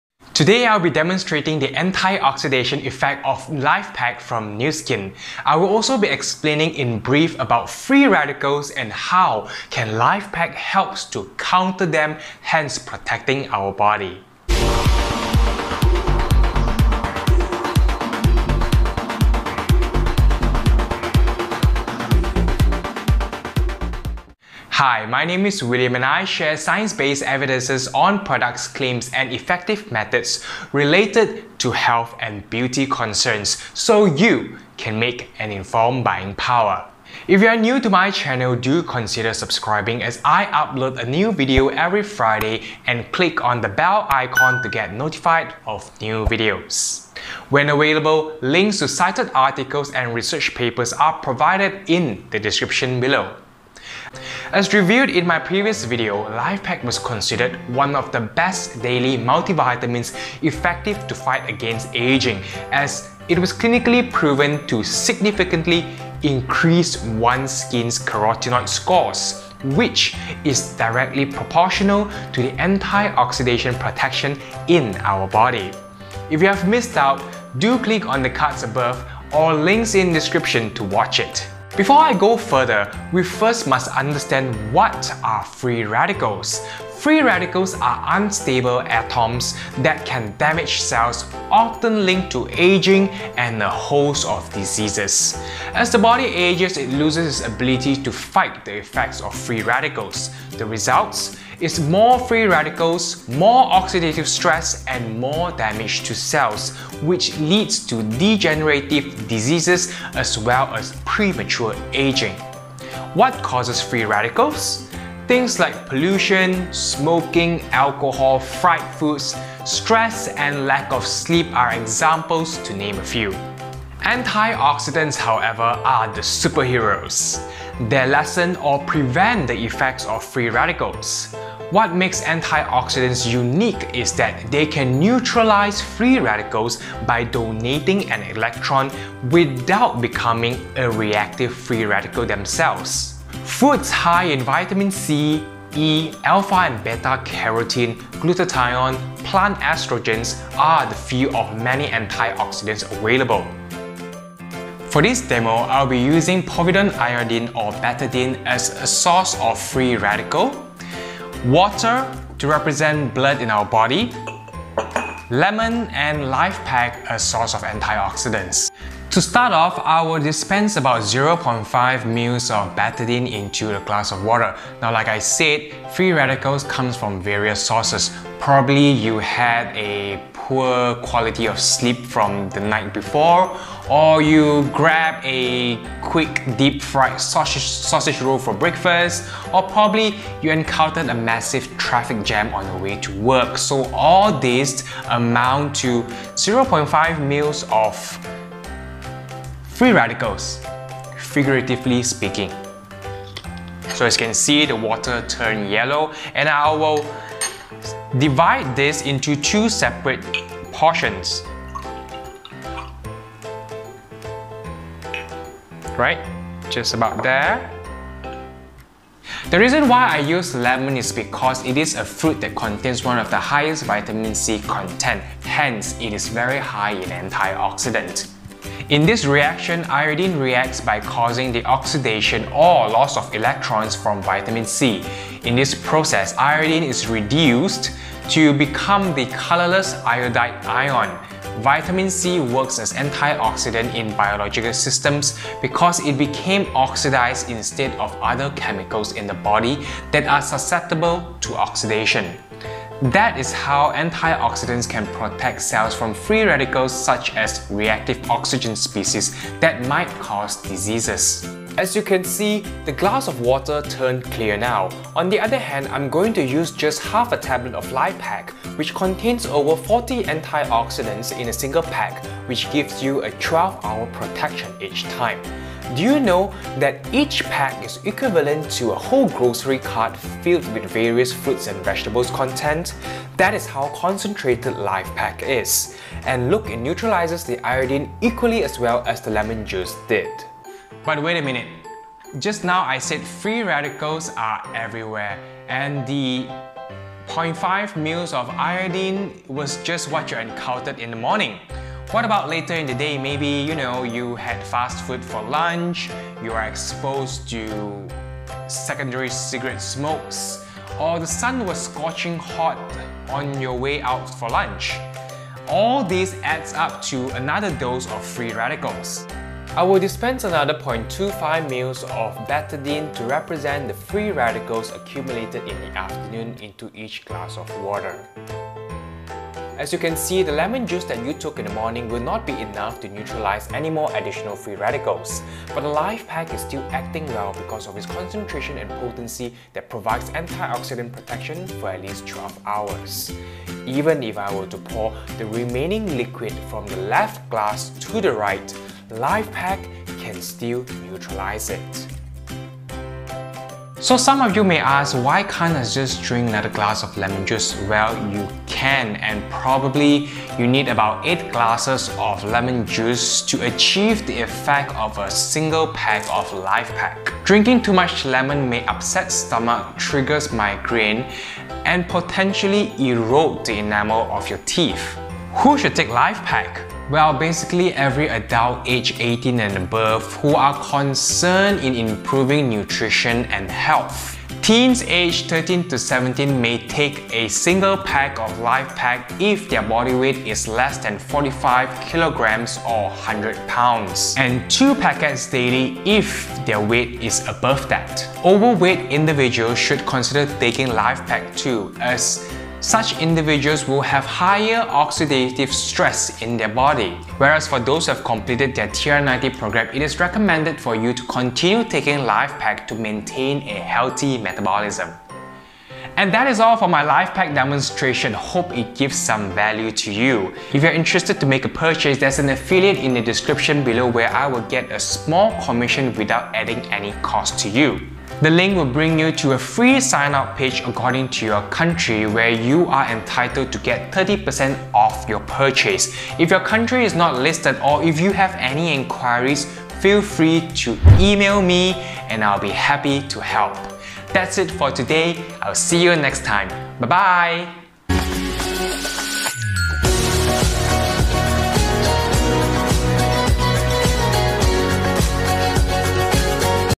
Antioxidant & Free Radical: Demo with Antioxidant High Supplement / Vitamin (Nu Skin LifePak)
[Video summary]
Understand what is antioxidants and free radicals in brief. How do supplements or vitamins rich in antioxidant like LifePak neutralizes the damaging effects of free radical? Here is a simple home demonstration of how LifePak protects our body. Product used is Nu Skin LifePak.

[Timestamps]
00:00 LifePak introduction
00:54 Recall previous LifePak nano and LifePak video content
01:23 What are free radicals?
02:04 What are antioxidants?
02:34 LifePak demo
06:15 Who should take LifePak?
06:56 Purchase link

[Reference(s)]
Previous video on Best multivitamins 2020 for adults and anti-aging? LifePak nano vs LifePak vs Amway vs Blackmores

Free sign up and purchase online according to your country. Successful sign ups will be entitled up to 30% discounts.

** I do not own or claim to own all the contents shown in my video.